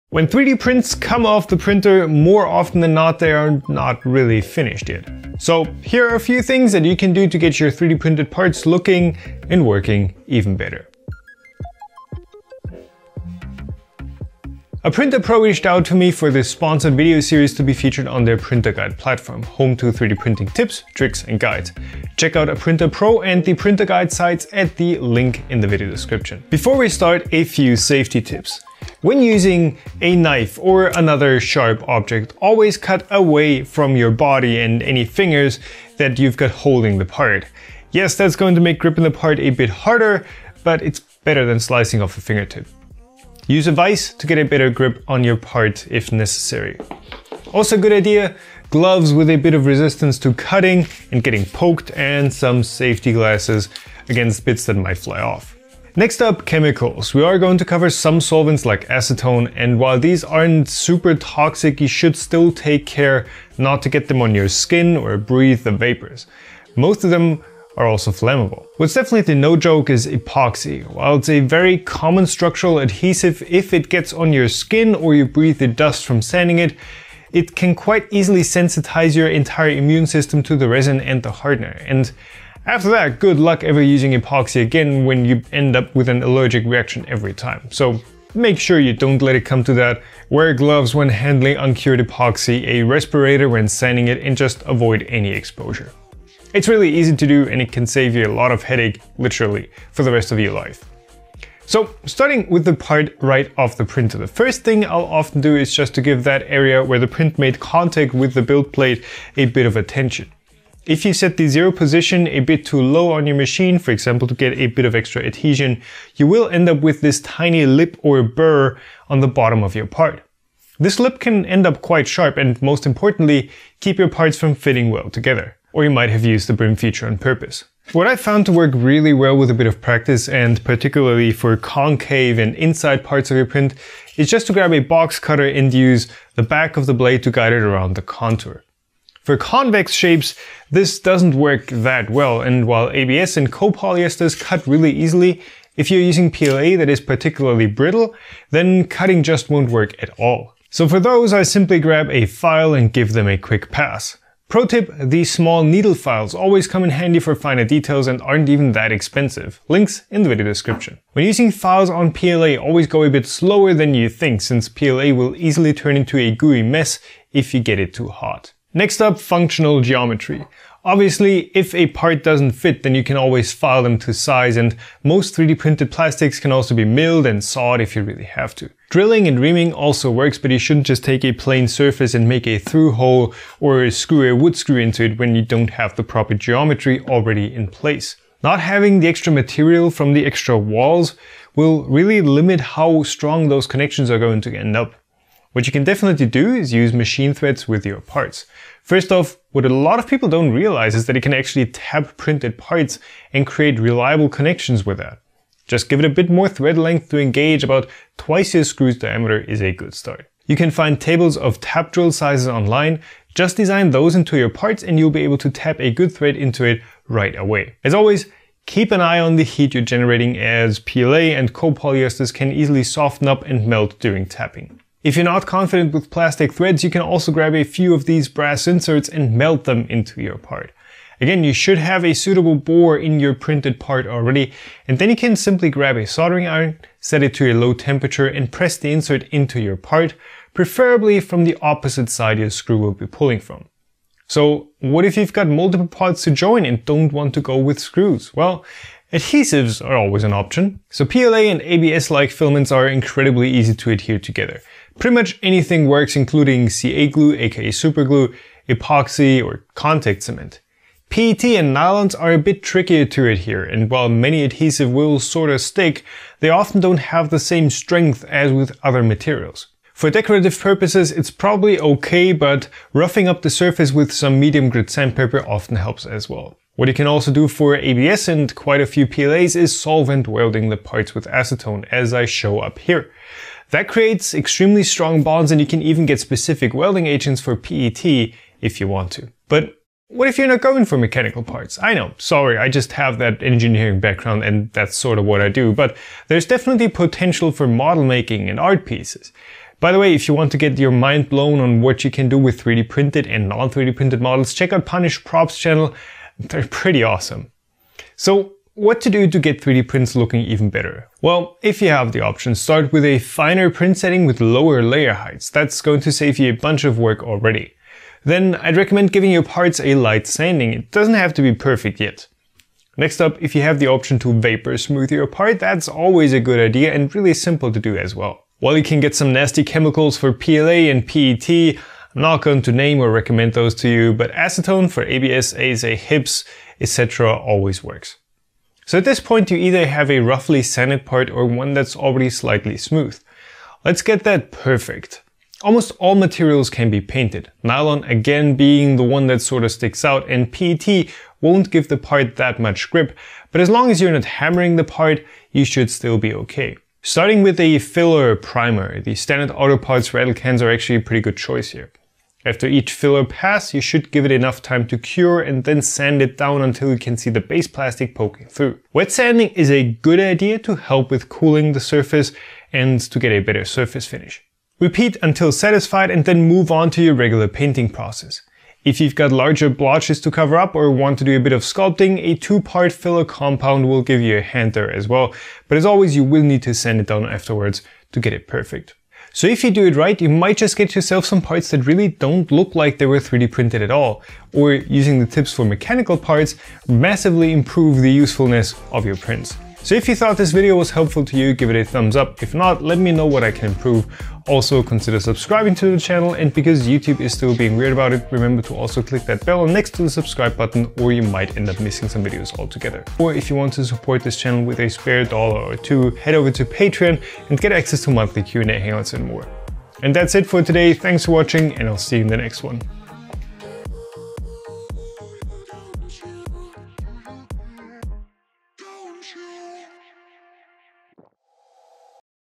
Basics: How to post-process your 3D prints!
A few simple steps can turn your 3D prints into something useful and special!